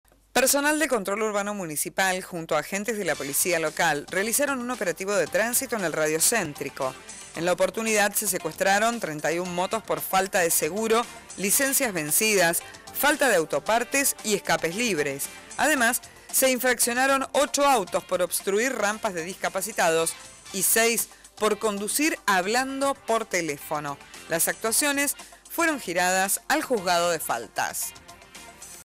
Continúan realizándose controles de tránsito
En el radio céntrico se llevaron adelante controles de tránsito donde se secuestraron vehículos y se realizaron infracciones.